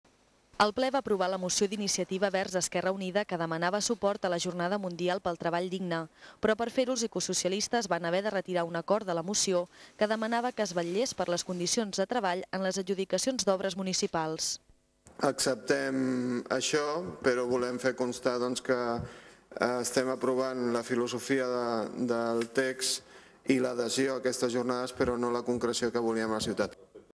Suport Jornada Mundial treball digne
Moció al ple municipal de setembre de 2008